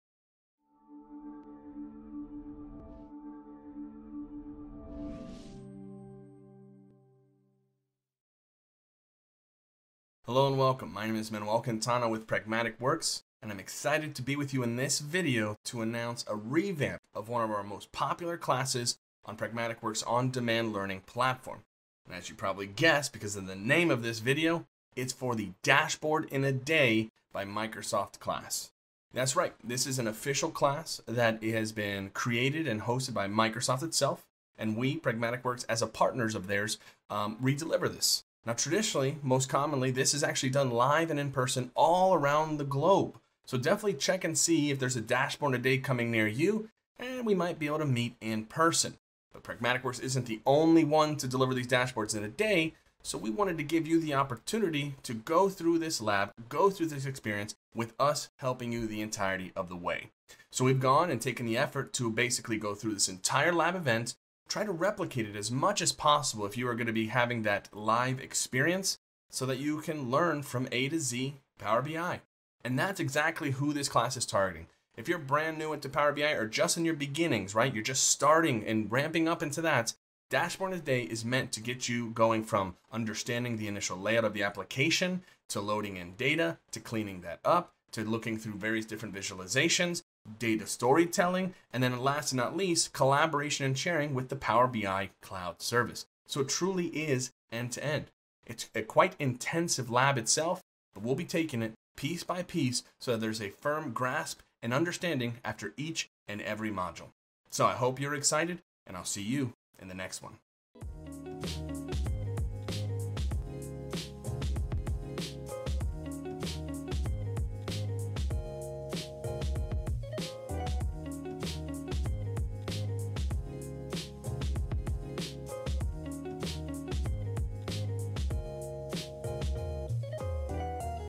Dashboard in a Day Course Updates
To ensure our Power BI content is current, we've updated our Microsoft Dashboard in a Day course. Sign up for your free On-Demand Learning trial: